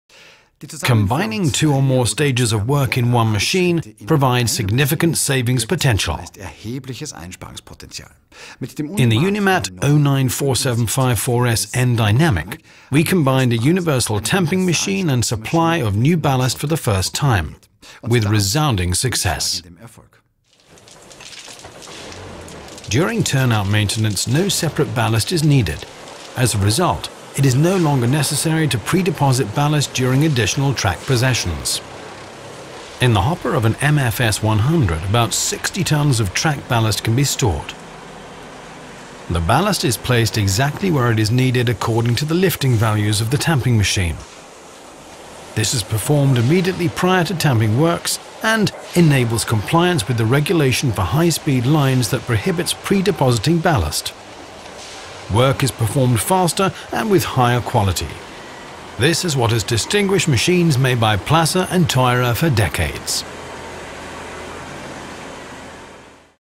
Turnout tamping with simultaneous supply of new ballast
Combining two or more stages of work in one machine provides significant savings potential.

In the Unimat 09-475/4S N-Dynamic, we combined a universal tamping machine and supply of new ballast for the first time – with resounding success.

During turnout maintenance, no separate ballast is needed. As a result, it is no longer necessary to pre-deposit ballast during additional track possessions.

In the hopper of a MFS 100 about 60 tonnes of track ballast can be stored.

The ballast is placed exactly where it is needed according to the lifting values of the tamping machine. This is performed immediately prior to tamping works and enables compliance with the regulation for high-speed lines that prohibits pre-depositing ballast.

Work is performed faster and with higher quality.

This is what has distinguished machines made by Plasser & Theurer for decades.